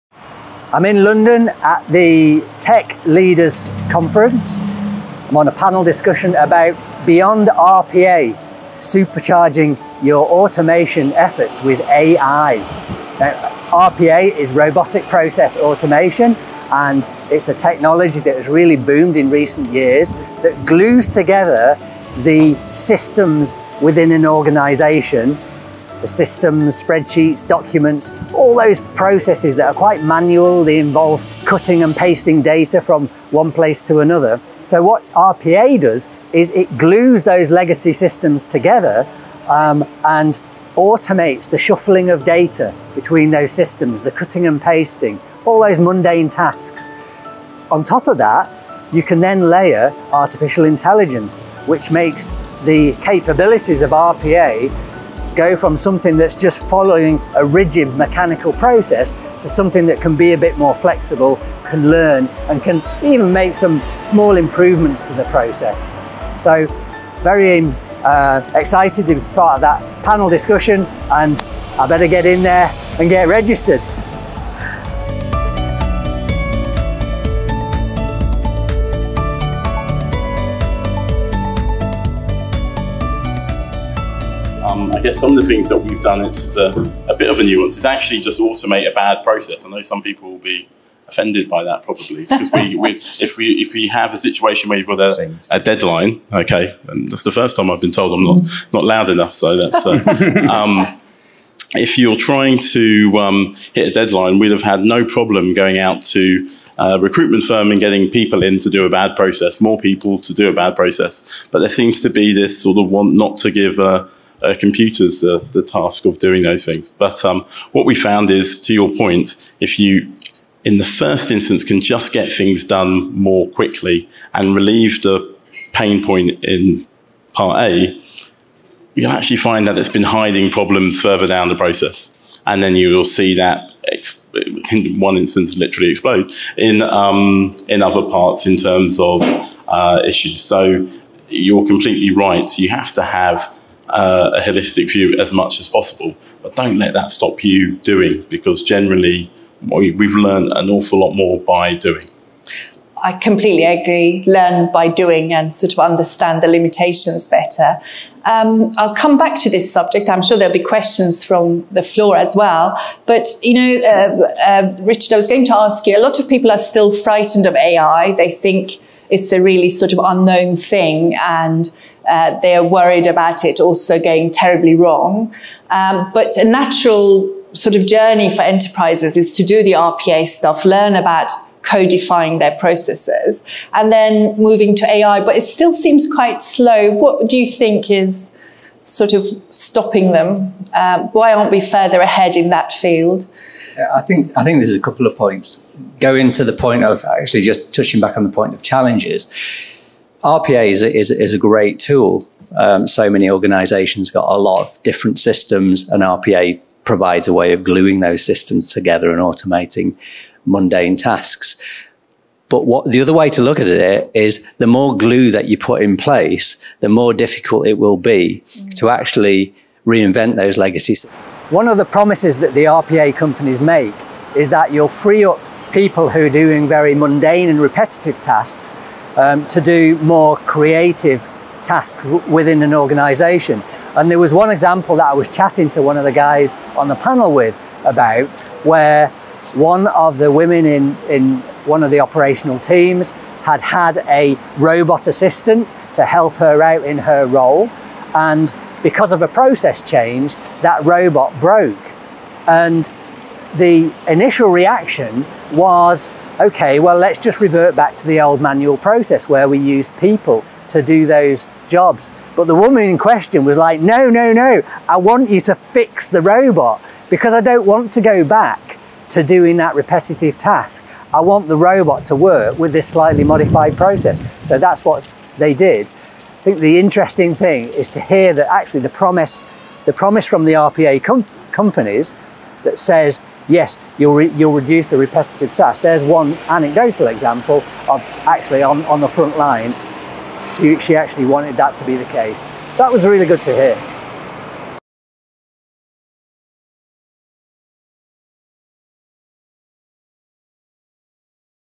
Richard's Blog - Episode 9: Using AI to automate your business processes
On 12 September 2019, I took part in a panel discussion on Beyond RPA: using AI to super-charge your automation efforts, as part of the Tech Leaders Summit in London.  RPA stands for Robotic Process Automation.  It’s a method of gluing together existing systems, spreadsheets, web portals, documents, paper forms, etc. by using a piece of software called a “robot” to move and process the data by simulating the key presses and mouse clicks that would normally be done by a human.  It’s particularly suited to high volume repetitive processes.

According to Gartner, RPA was the fastest growing segment of the enterprise software market in 2018, growing 63% to $846m (£680m), and expected to grow to $1.3bn (£1bn) in 2019.

Advances in AI are allowing the “robots” to become more sophisticated and take on more complex tasks.  Our panel discussion covered the current practicalities of using RPA and then looked into the challenges and opportunities that AI presents for the future.

Will the Terminator be processing your next insurance renewal!? 